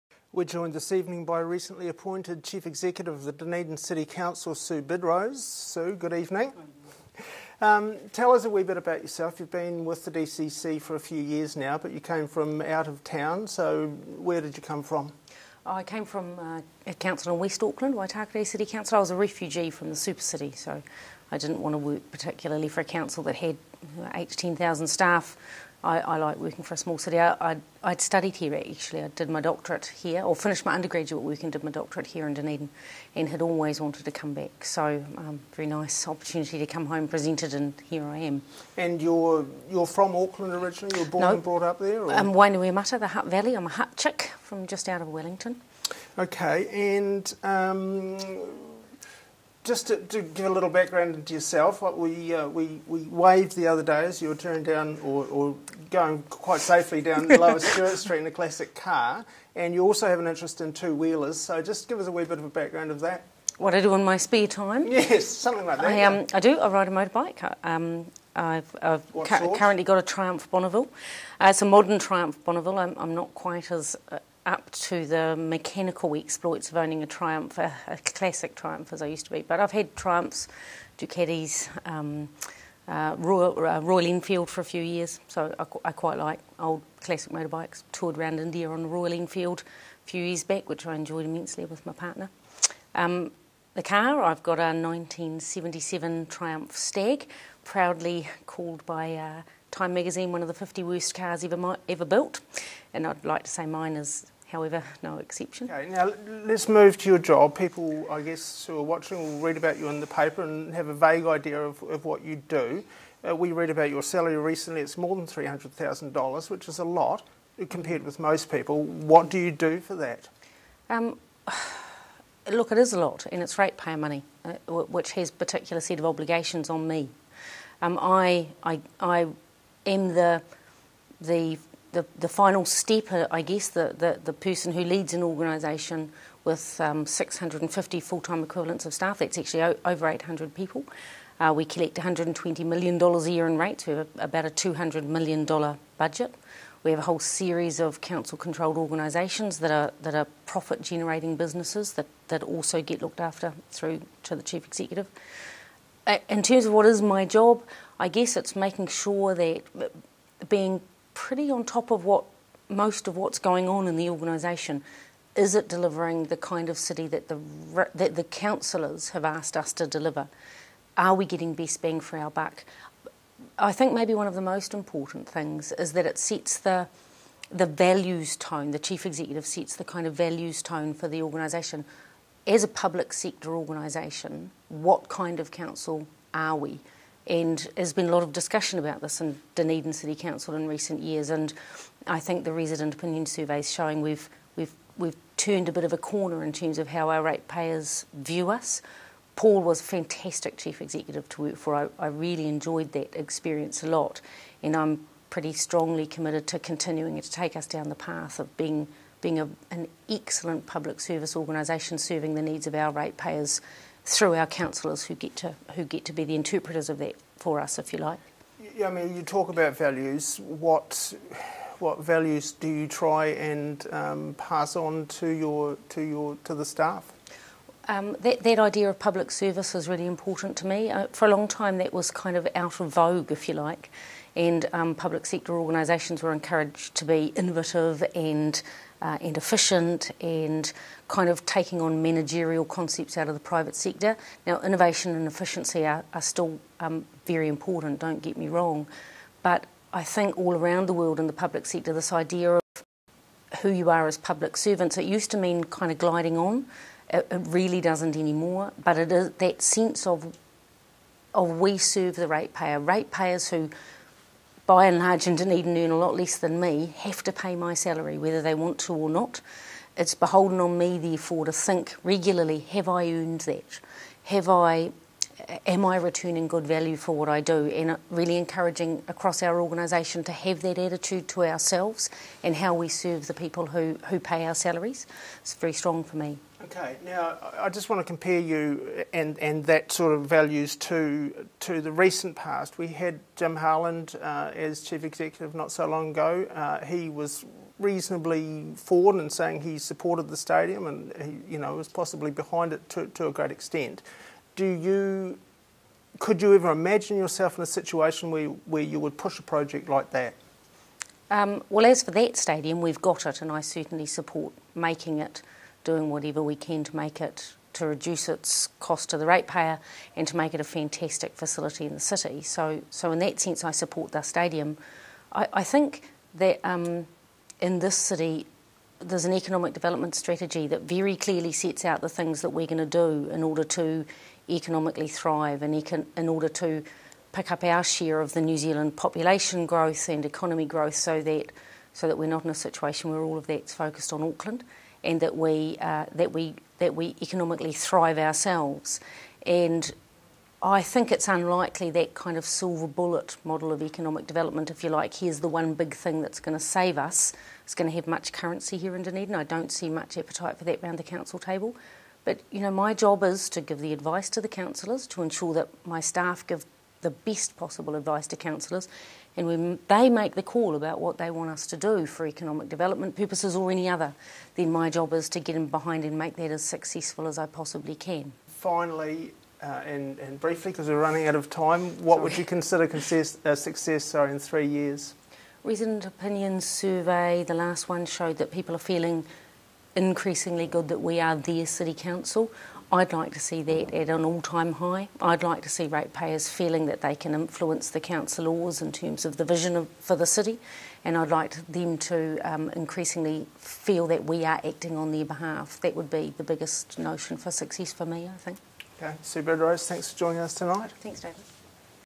Sue Bidrose
The Dunedin City Council had some turnover at the top late this year, after chief executive Paul Orders announced he was moving back to Wales after a two year stint. Order's time at the top was widely acknowledged as a period in which the council's finances were put in order. The search to replace him resulted in a promotion from within. Sue Bidrose took on the role in charge of more than 600 staff who run everything from rubbish collection to civil defence to economic development. She came into the studio for a chat with chief reporter David Loughrey on subjects from motorbikes to stadiums to public service.